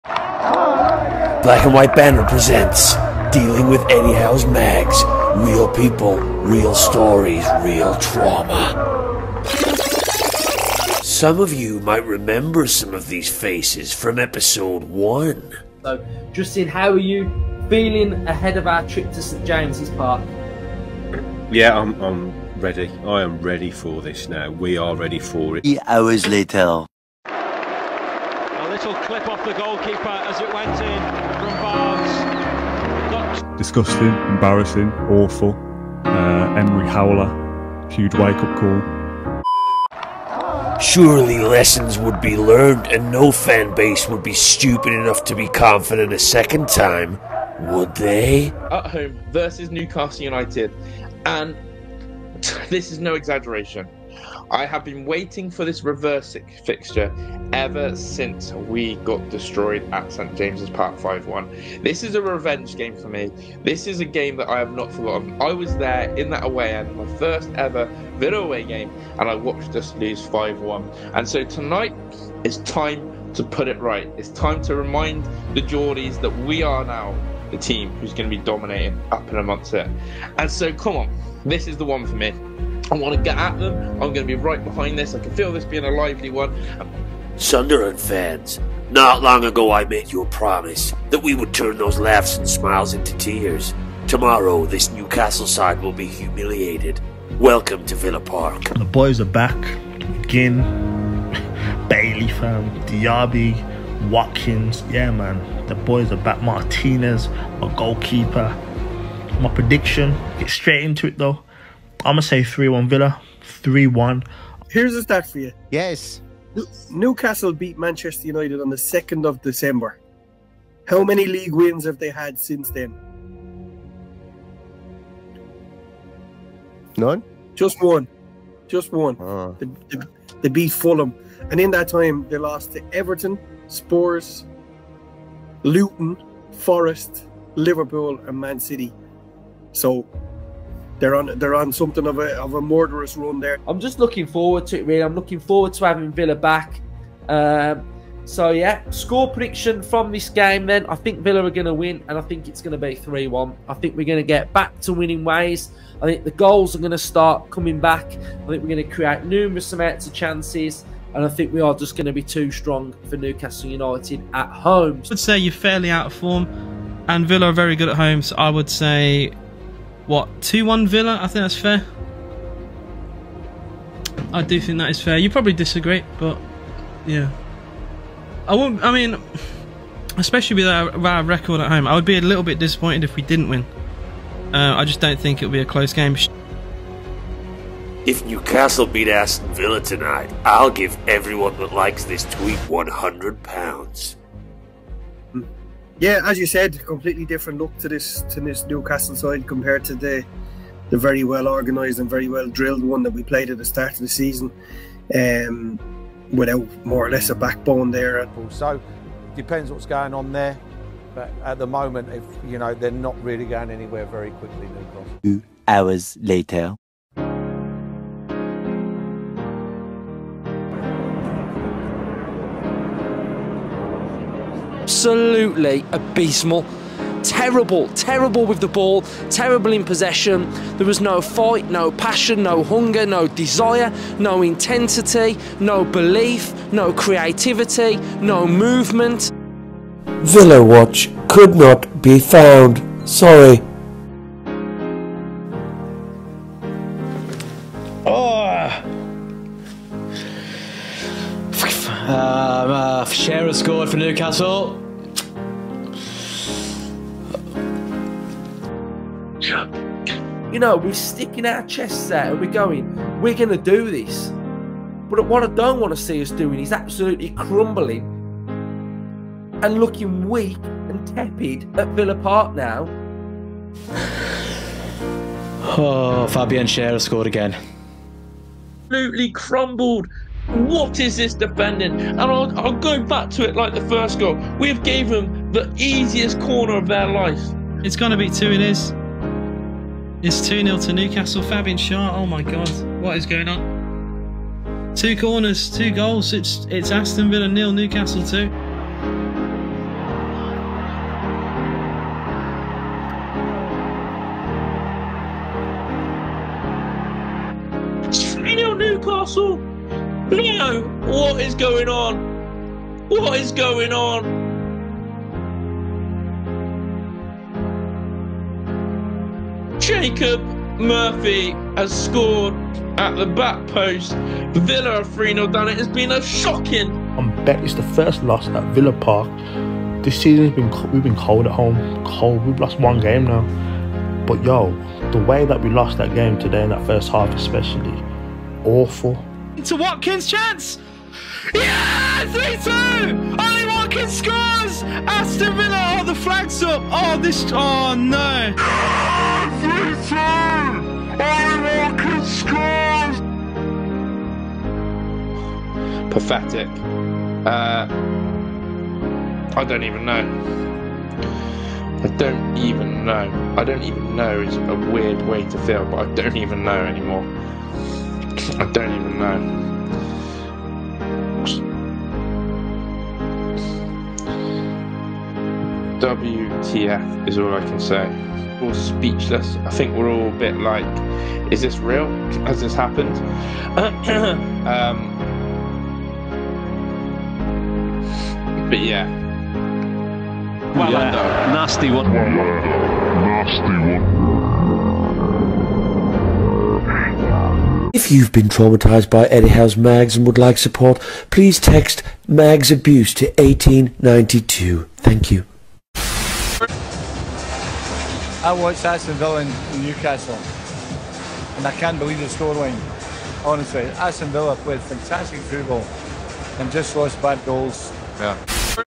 DEALING WITH EDDIE HOWE’S MAGS | *Aston Villa vs Newcastle*
Time to sit back and enjoy another episode of Dealing with Eddie Howe’s Mags as Aston Villa fans pay the price for being a little too confident ahead of the visit of Newcastle United.